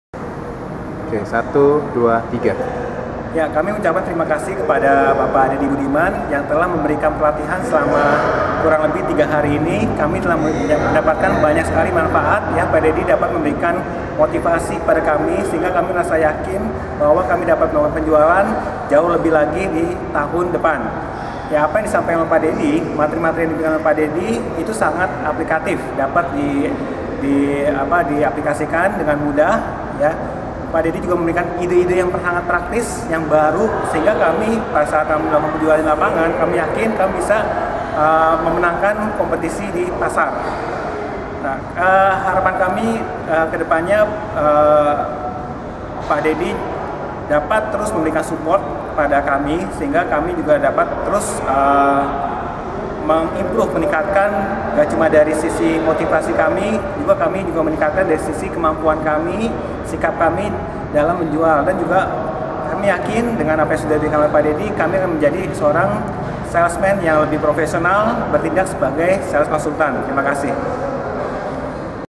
Testimony Pieter Andre untuk Dedy Budiman
Pieter Andre -  Hino Sales Training Center Dept. Head at PT. Hino Motors Sales Indonesia memberikan kesan terhadap Dedy Budiman.

Materi Training yang diberikan Dedy Budiman aplikatif dan mudah dipahami oleh sales team Hino.

dedy budiman
sales trainer
HINO
sales training